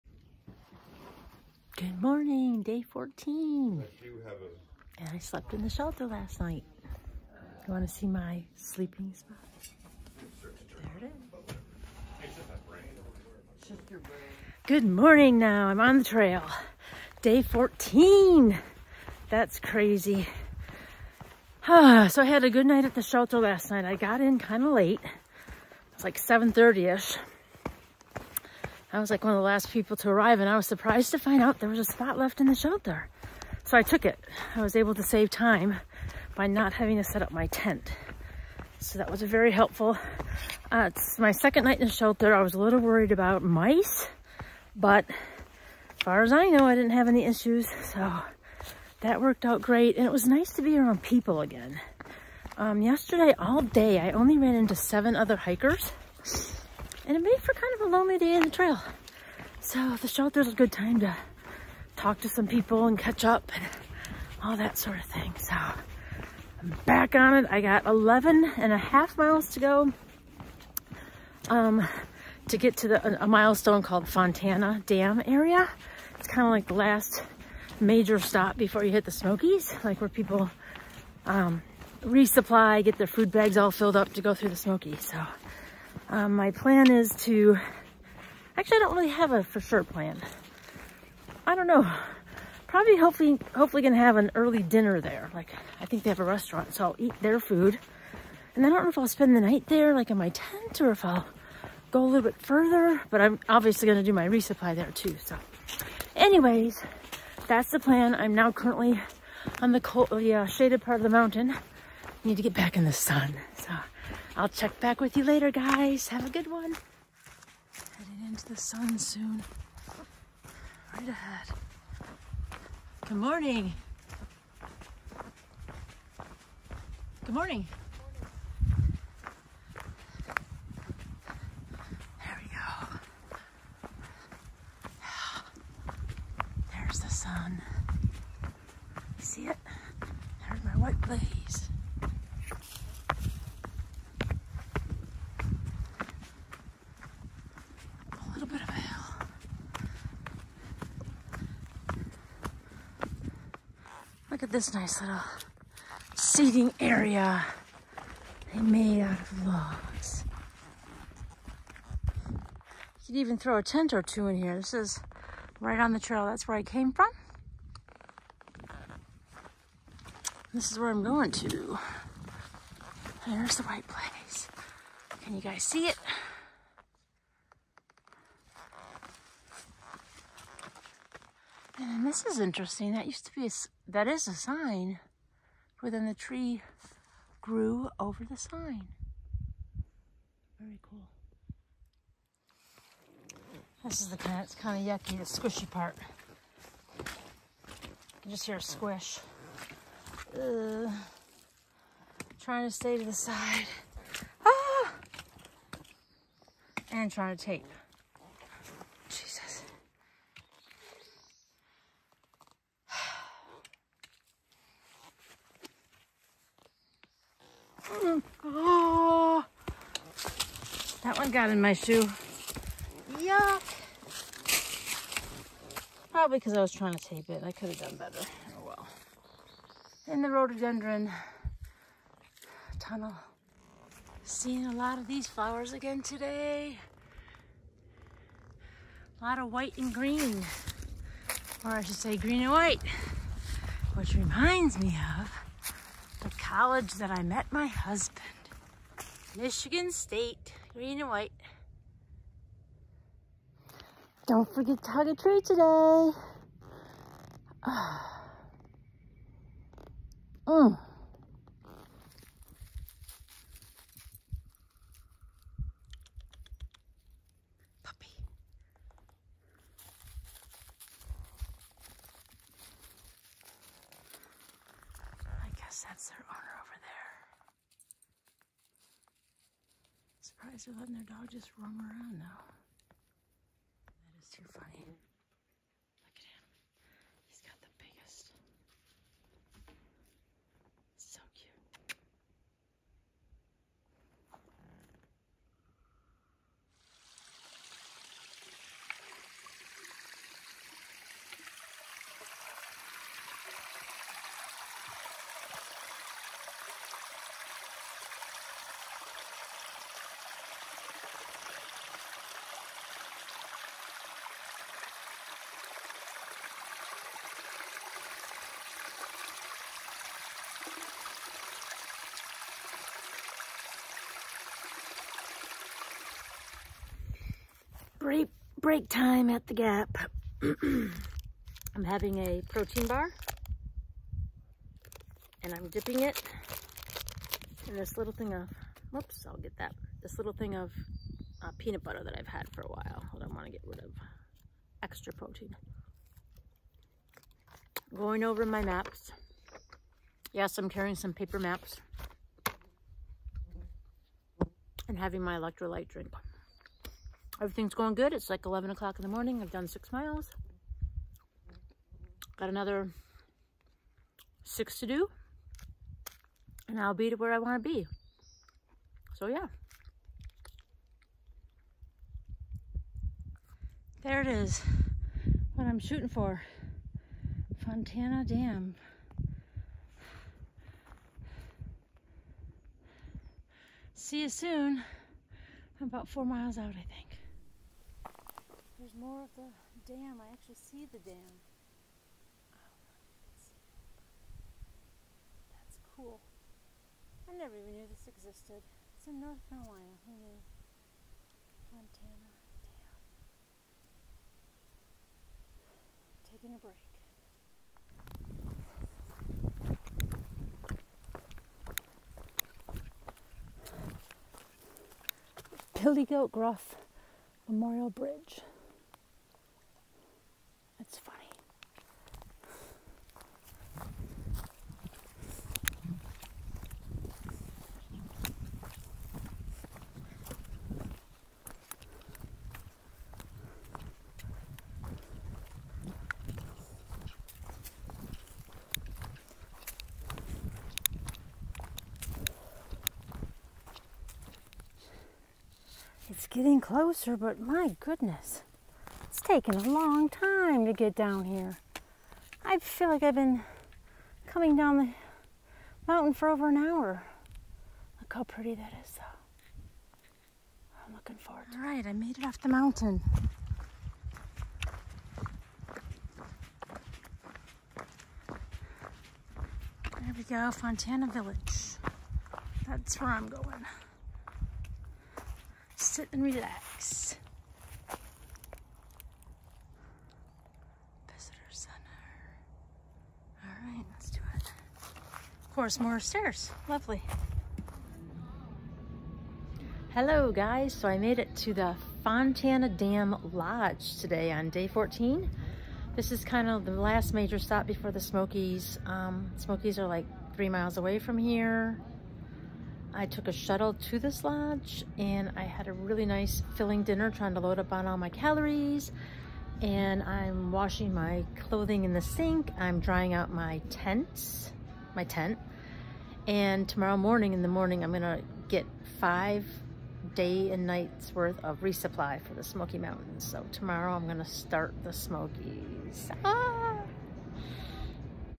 Day 14 on the Appalachian Trail
Just a personal video log of my adventures on Thru Hiking the Appalachian Trail. Any thoughtful tips are appreciated.